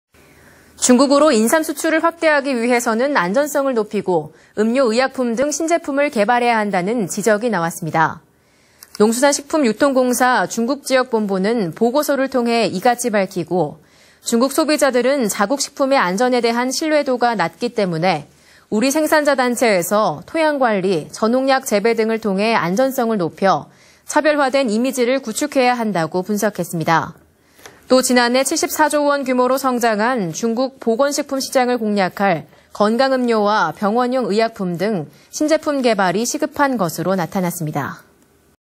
[안동MBC뉴스]"인삼 안전성 높이고 신상품 개발해야"
2016/09/13 09:58:59    작성자 : 김건엽

중국으로 인삼 수출을 확대하기 위해서는
안전성을 높이고 음료·의약품 등
신제품을 개발해야 한다는 지적이 나왔습니다.
◀ＥＮＤ▶

농수산식품유통공사 중국지역본부는
보고서를 통해 이같이 밝히고
중국 소비자들은 자국 식품의 안전에 대한
신뢰도가 낮기 때문에 우리 생산자단체에서
토양관리·저농약재배 등을 통해 안전성을 높여
차별화된 이미지를 구축해야 한다고
분석했습니다.

　또 지난해 74조원 규모로 성장한
중국 보건식품 시장을 공략할
건강음료와 병원용 의약품 등 신제품 개발이
시급한 것으로 나타났습니다.